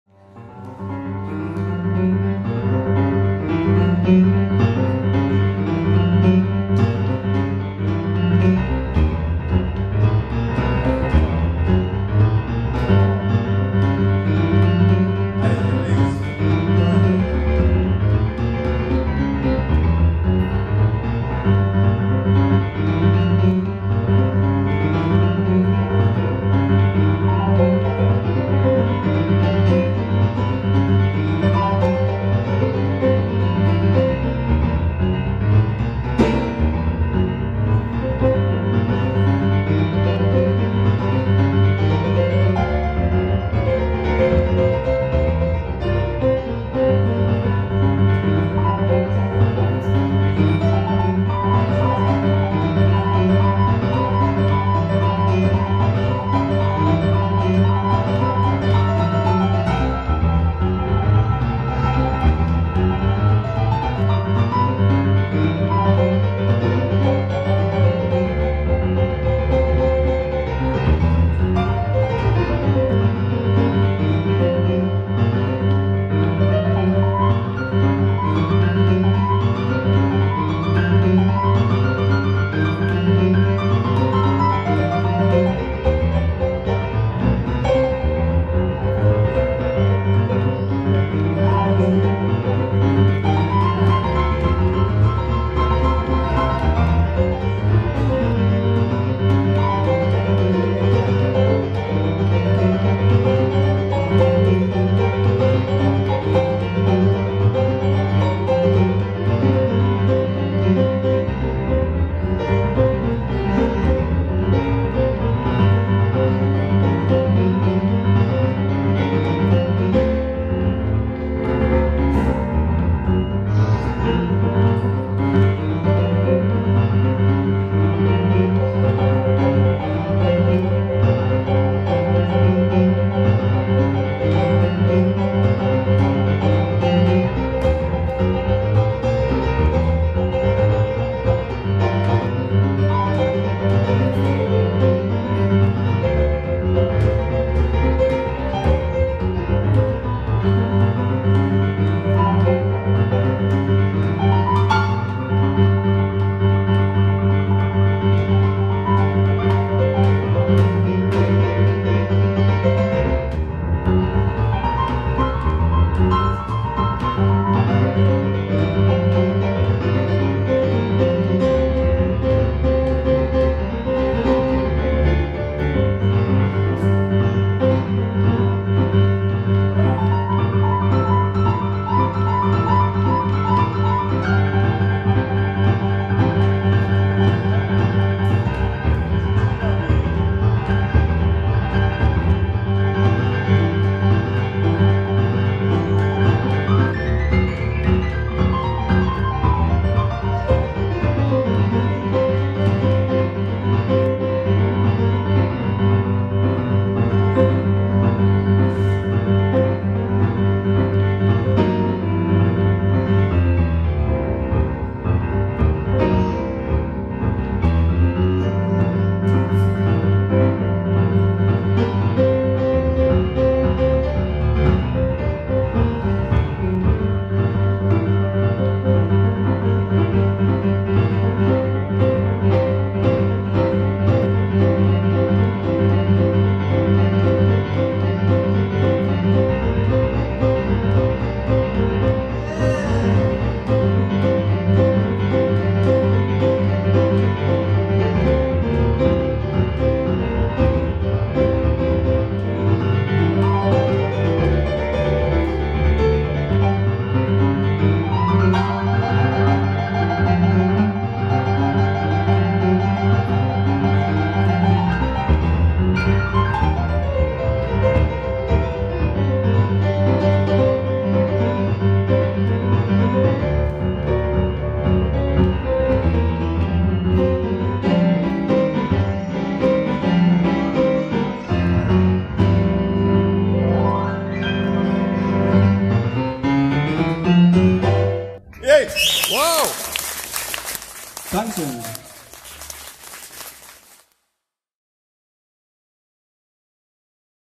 Luca Sestak - Boogie Woogie Piano - Pinneberger Summerjazz 2012
Luca Sestak (17) - Aufgenommen beim Pinneberger Summerjazz 2012